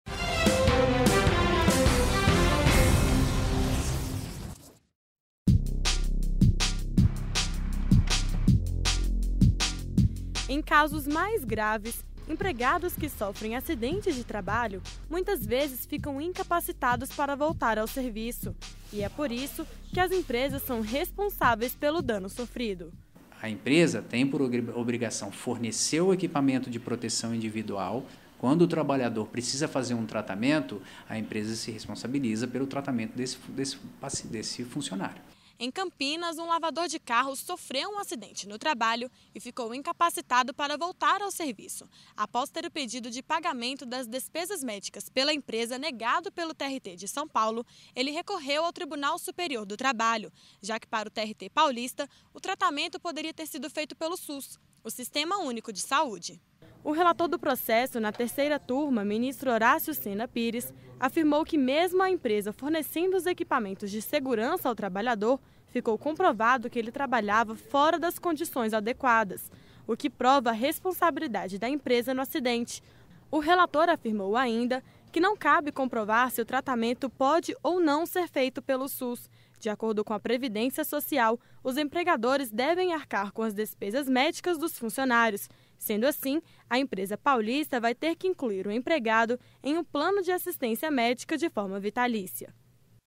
Trabalhador acidentado não precisa provar que tratamento médico não é feito pelo SUS
A Terceira Turma do Tribunal Superior do Trabalho condenou a empresa Porto Vitória Veículos Ltda. a custear as despesas médicas de um empregado incapacitado para o trabalho devido a limitações do aparelho locomotor depois de sofrer acidente de trabalho, mesmo sem ele ter comprovado que seu tratamento de saúde não podia ser realizado pelo Sistema Único de Saúde -- SUS. O relator do processo, ministro Horácio de Senna Pires, destacou que a comprovação exigida do trabalhador não tem respaldo em lei.